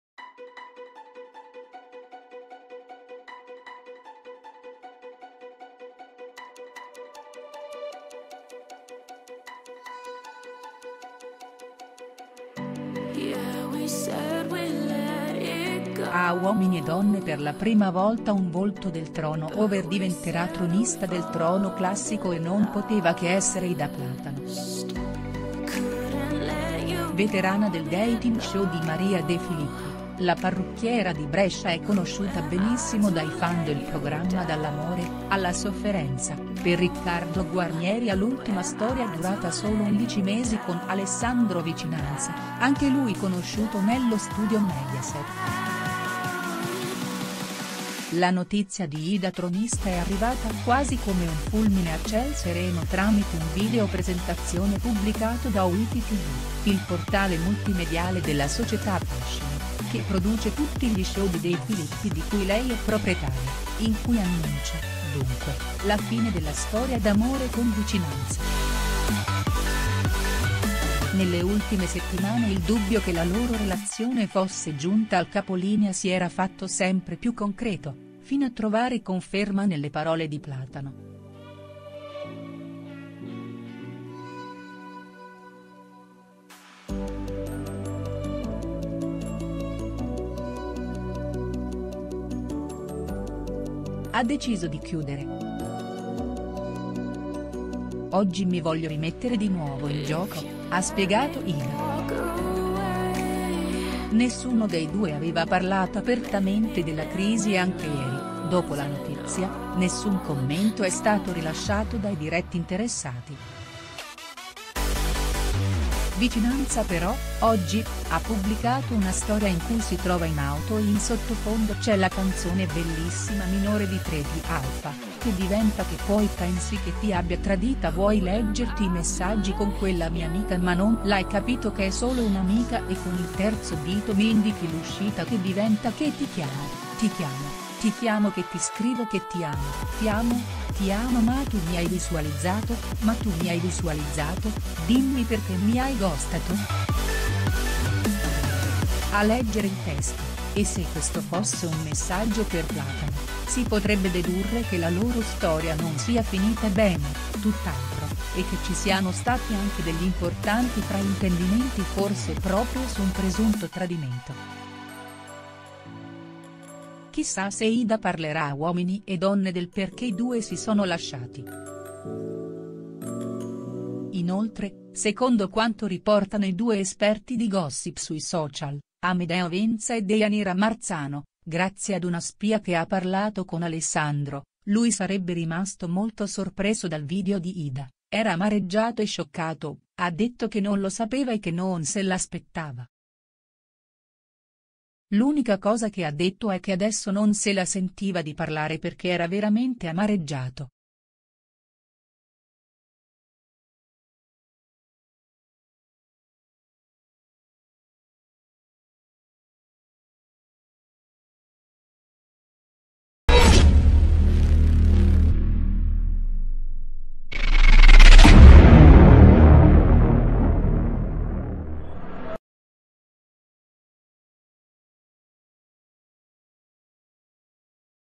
Ida a Uomini e Donne, il retroscena su Alessandro Vicinanza: "Non sapeva nulla"
Ida a Uomini e Donne, il retroscena su Alessandro Vicinanza: "Non sapeva nulla" A Uomini e Donne per la prima volta un volto del 'trono over' diventerà tronista del 'trono classico' e non poteva che essere Ida Platano. Veterana del dating show di Maria De Filippi, la parrucchiera di Brescia è conosciuta benissimo dai fan del programma dall'amore, alla sofferenza, per Riccardo Guarnieri all'ultima storia durata solo 11 mesi con Alessandro Vicinanza, anche lui conosciuto nello studio Mediaset. La notizia di Ida tronista è arrivata quasi come un fulmine a ciel sereno tramite un video-presentazione pubblicato da Witty tv, il portale multimediale della società Fascino (che produce tutti gli show di De Filippi di cui lei è proprietaria), in cui annuncia, dunque, la fine della storia d'amore con Vicinanza. Nelle ultime settimane il dubbio che la loro relazione fosse giunta al capolinea si era fatto sempre più concreto, fino a trovare conferma nelle parole di Platano. "Ha deciso di chiudere